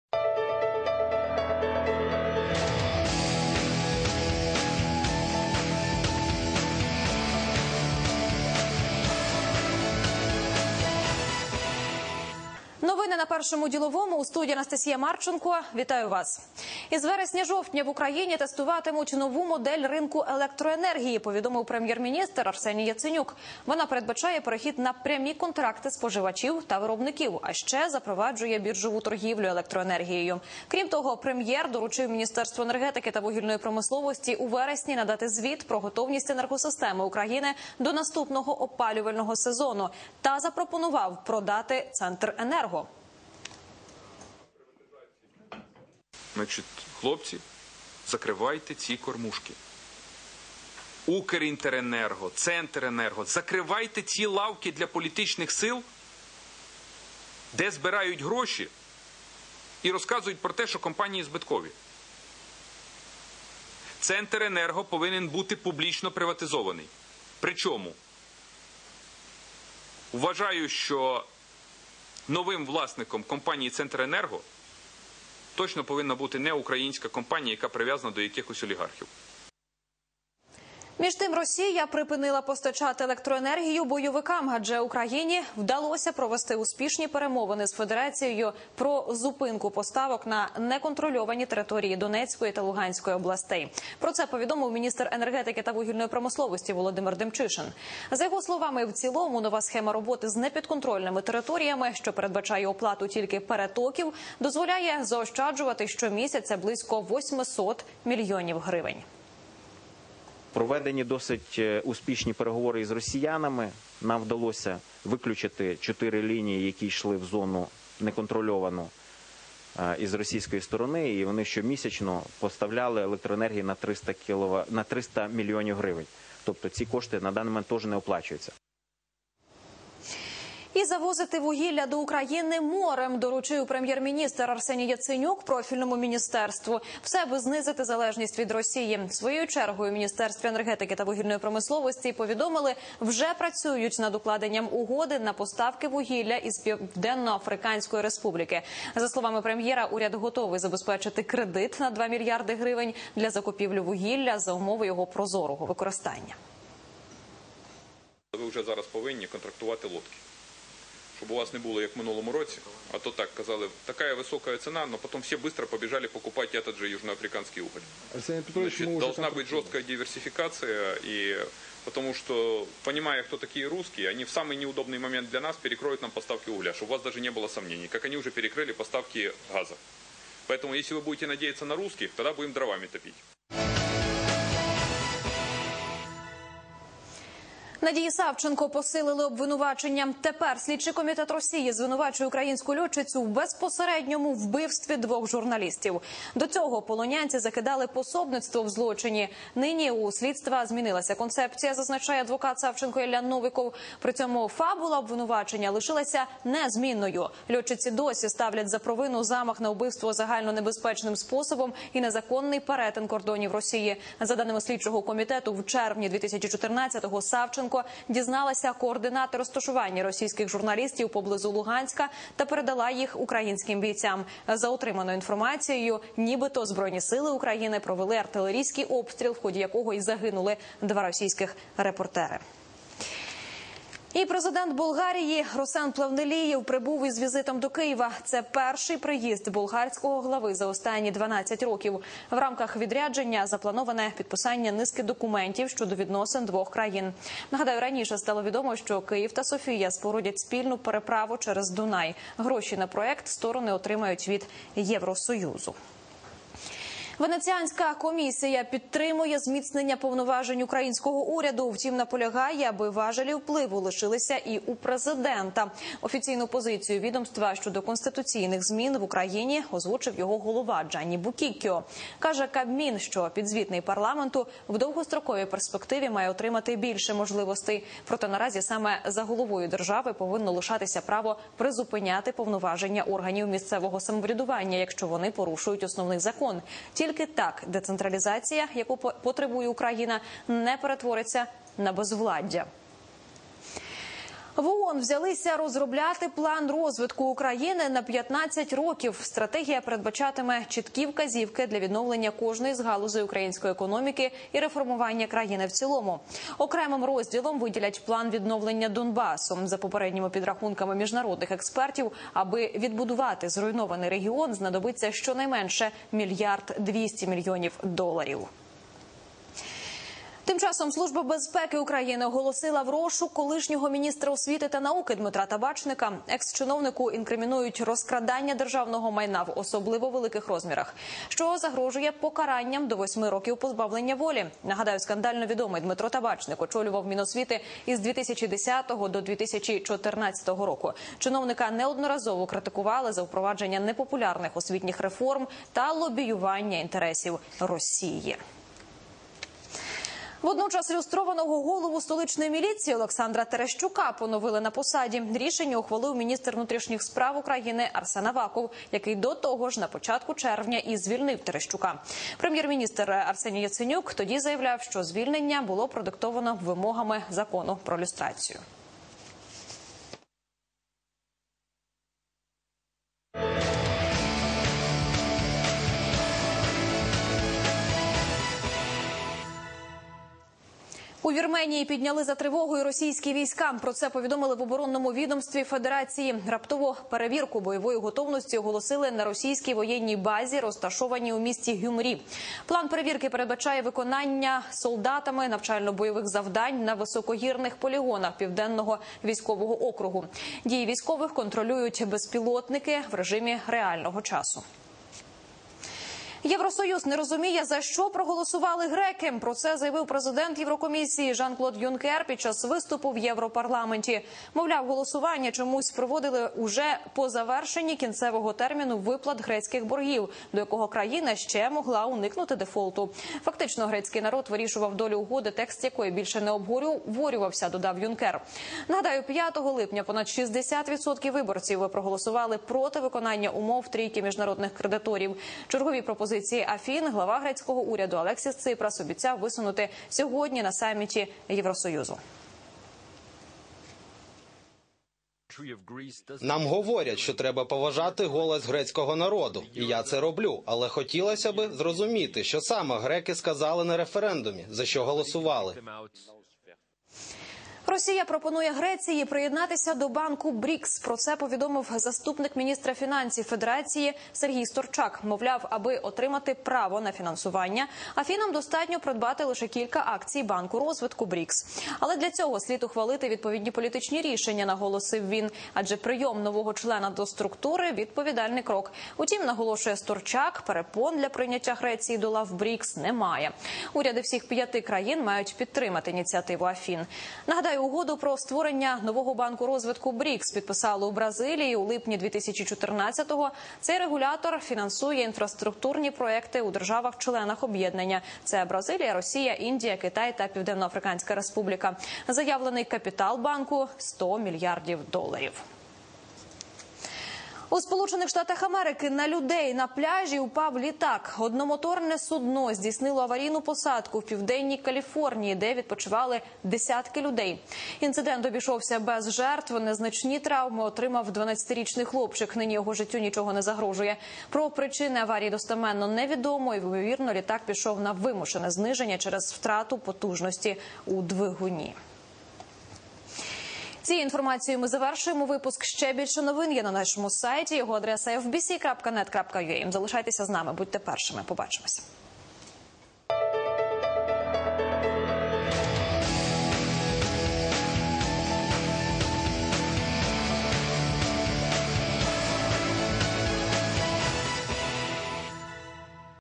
Осенью в Украине заработает новый рынок электроэнергии
Новости на "Первом деловом"